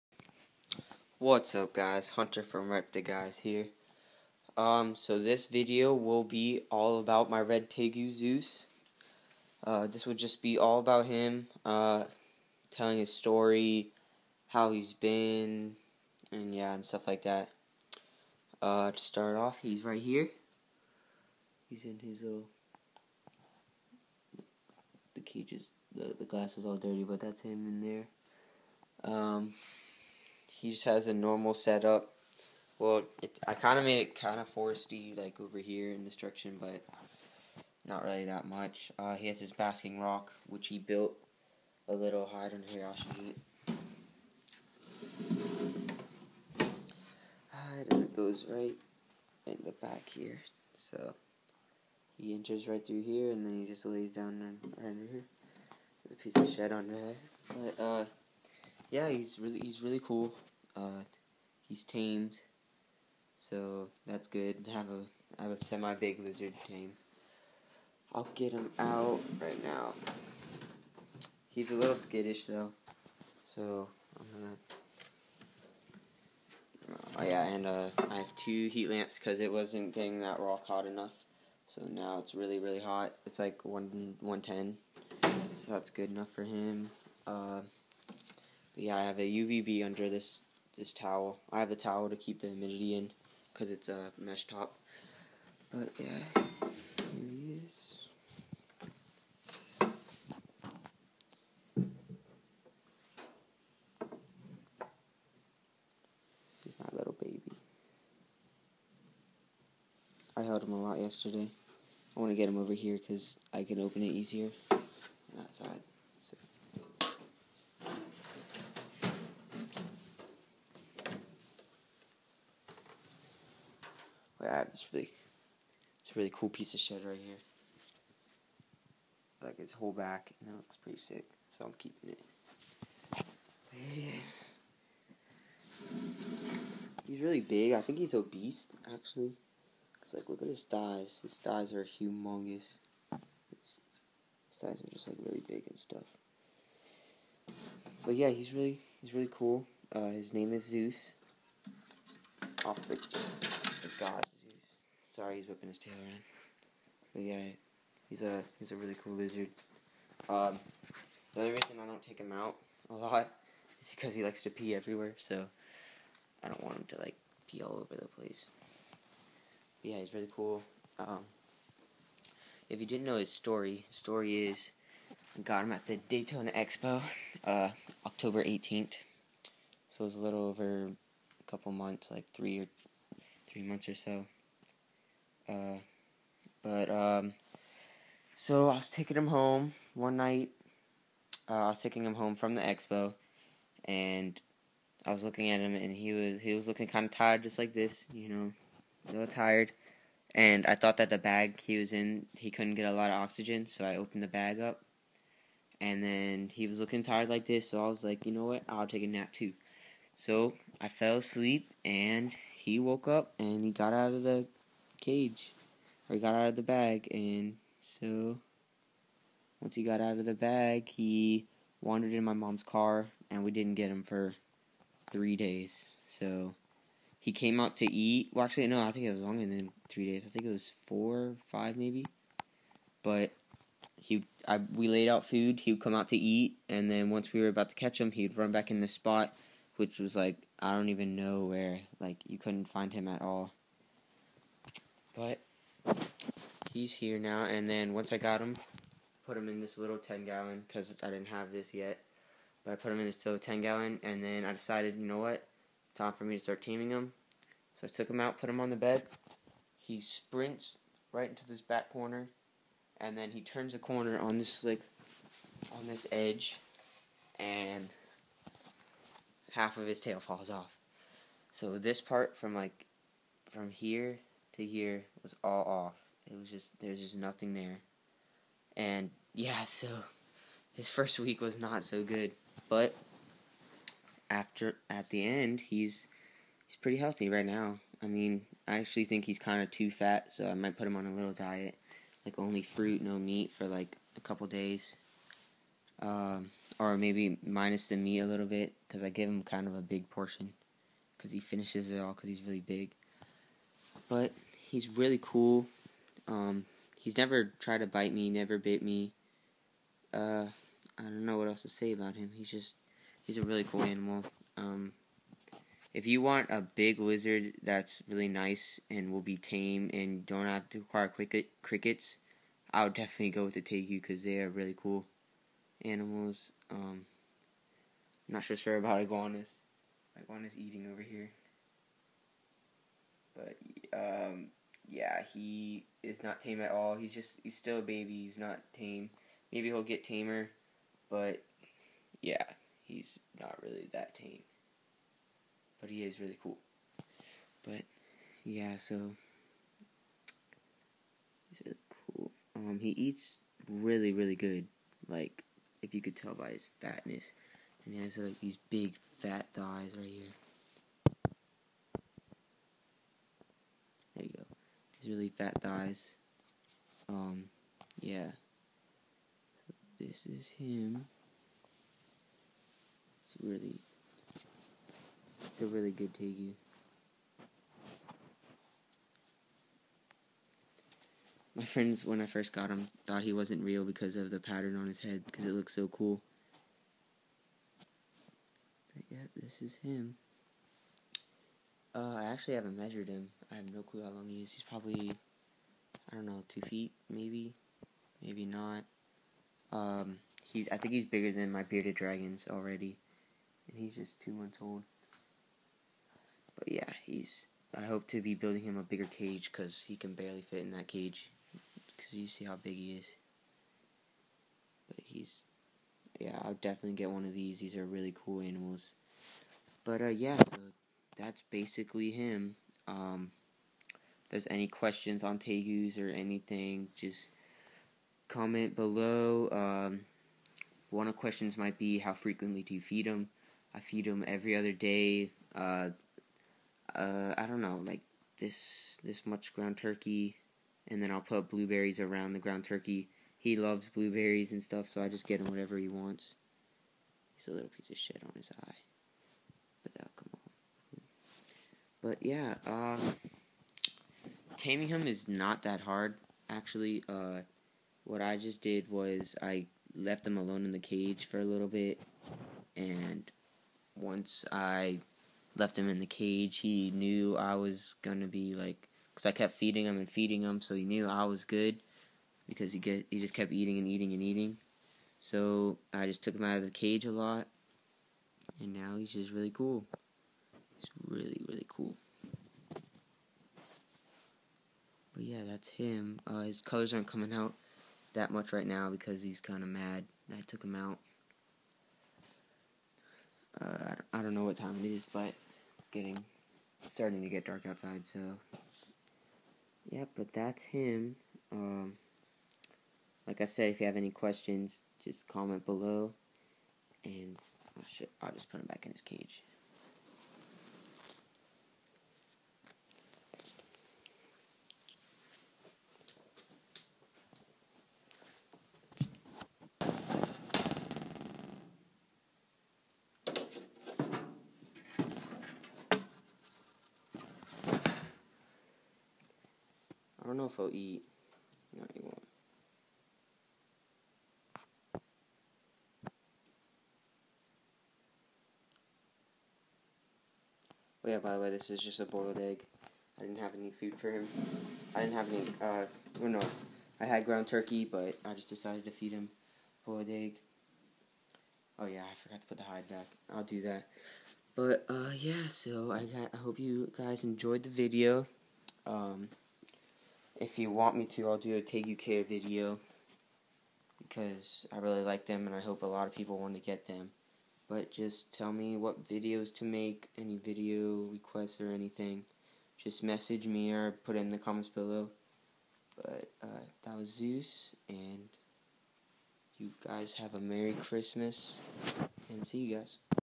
All About: My Red Tegu
This is a video about my tegu I hope you Guys enjoy and if you did leave a like and maybe favorite and i you have any questions just leave it below! If you like the All Abouts I will make more